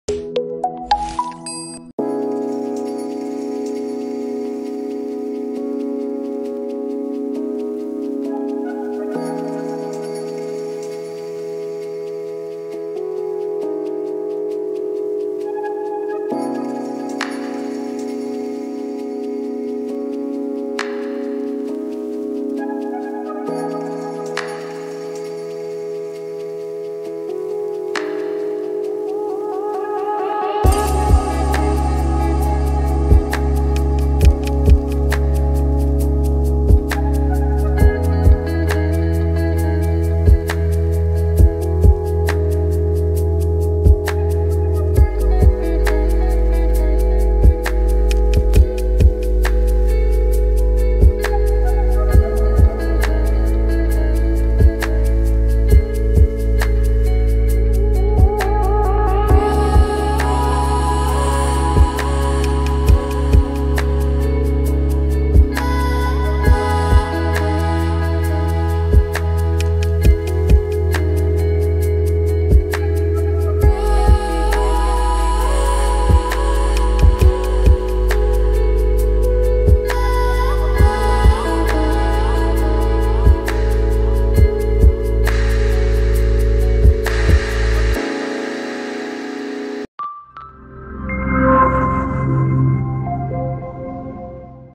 Corner Floor Lamp with 68 Dynamic Light Modes丨BlitzWolf BW-FLT1 - Banggood New Tech
Hello everyone, welcome to Banggood New Tech channel, I will show the RGB Colorful Lighting Effect of Corner Floor Lamp in this video, it has 68 Dynamic Light Modes, Stunning lights make your night full of fun.